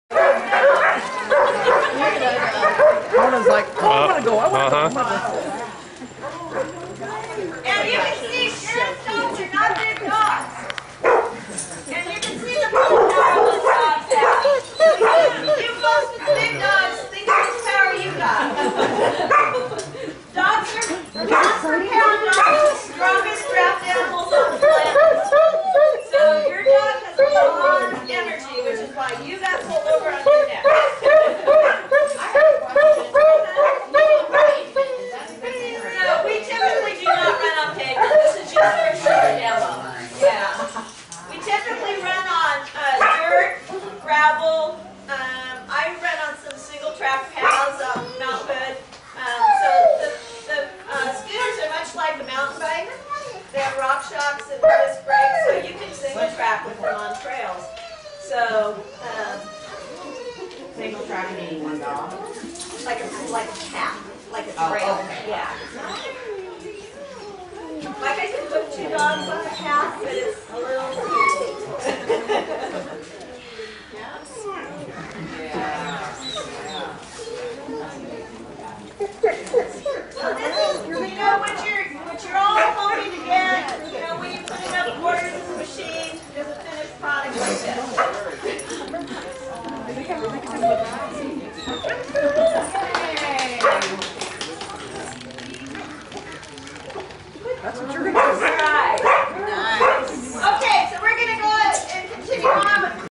Dog Scooter Demo
Demonstration of working dogs pulling a Diggler Scooter at the 2012 Fall Pull Clinic hosted by the Cascade Sled Dog Club.